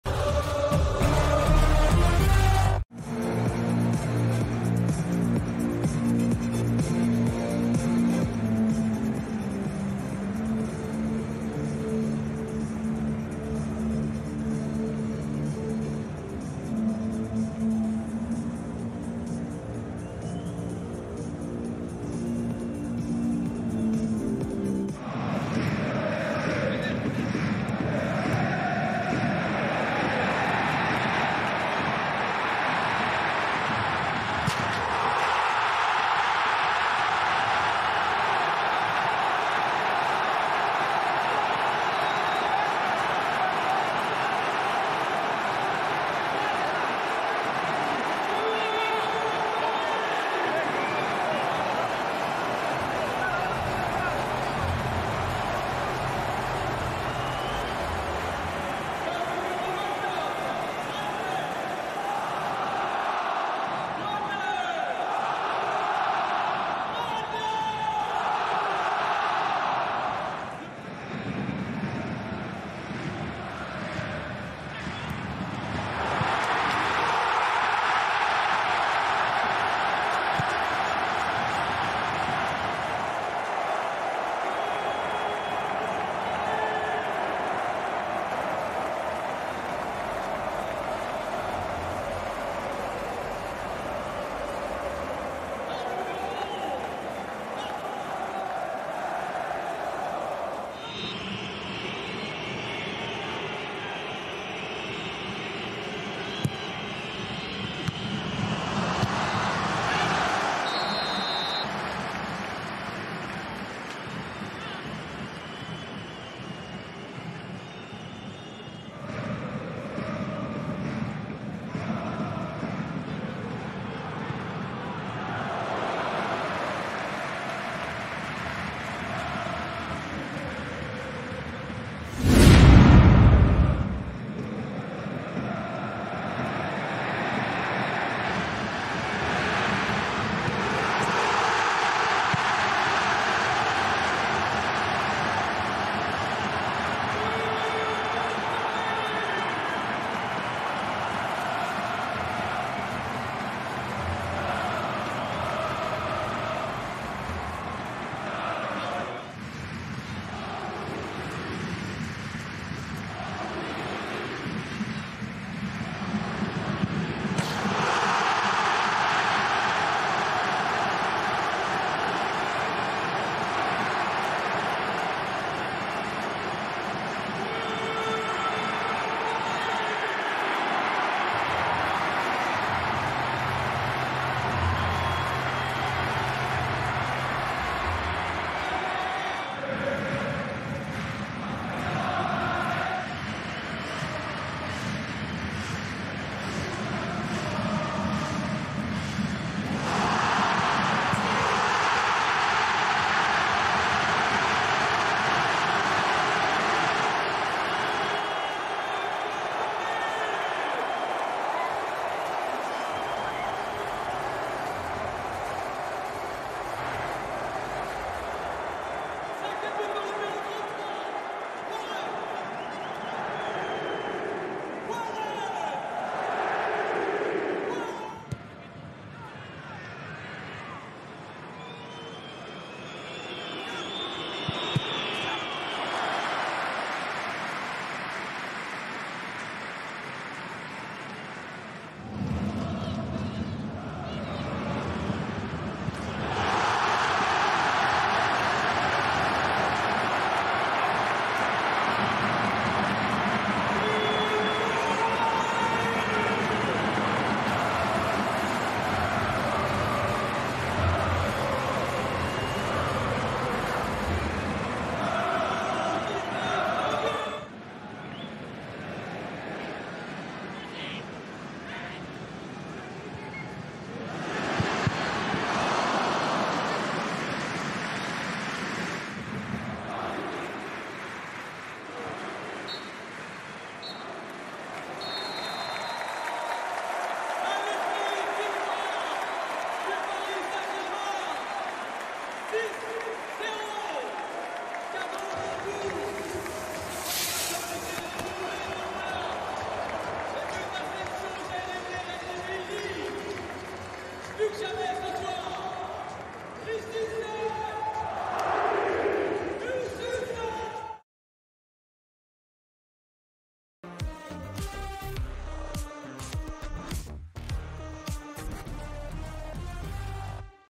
PARIS SAINT-GERMAIN - MONTPELLIER HÉRAULT SC (6-0) - 2ème journée - Ligue 1 McDonald's 24/25
Revivez les meilleurs moments de PARIS SAINT-GERMAIN - MONTPELLIER HÉRAULT SC (6-0) en vidéo.

Ligue 1 McDonald's  - Saison 2024/2025 - 2ème journée PARC DES PRINCES - Vendredi 23 Août 2024